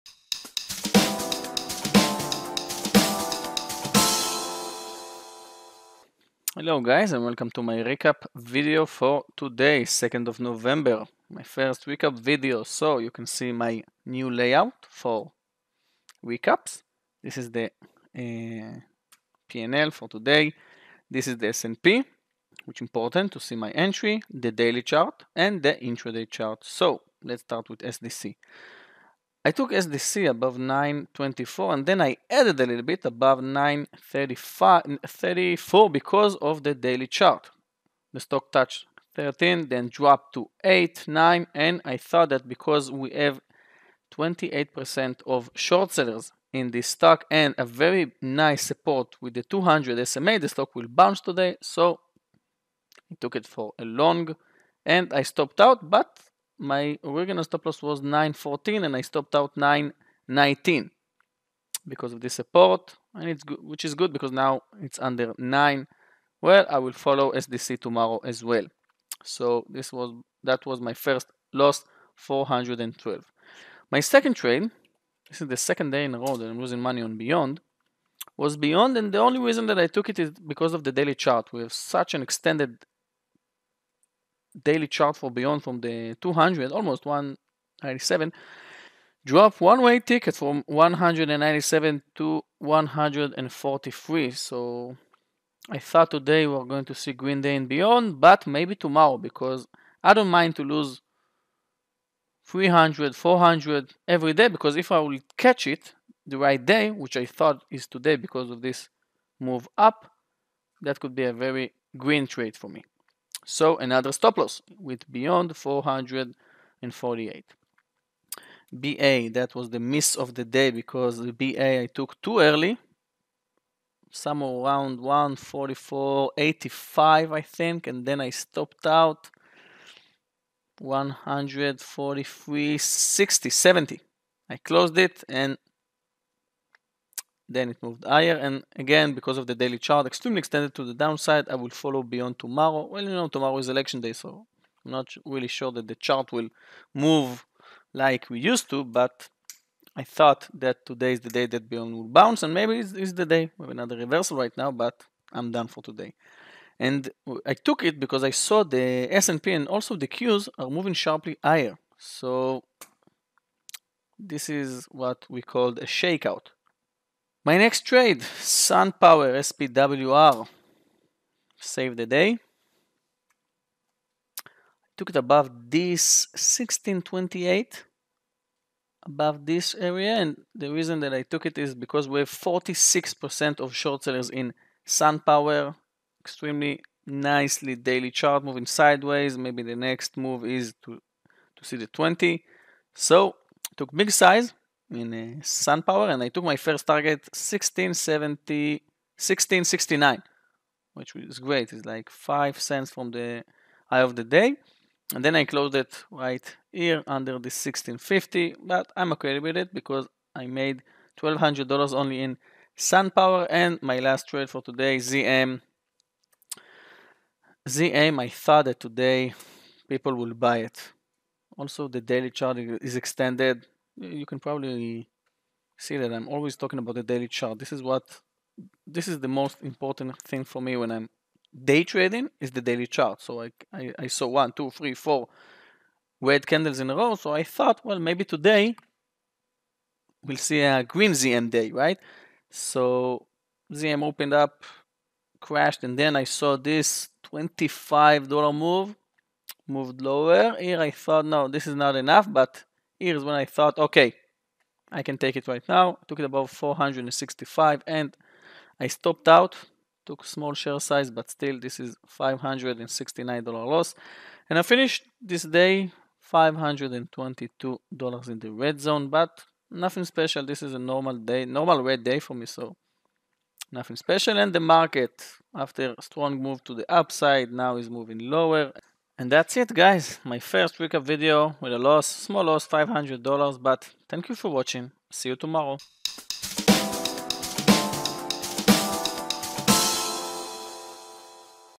Big miss on BA $552- Daily recap 11/2
My daily trades for 11/2/2020
Down $552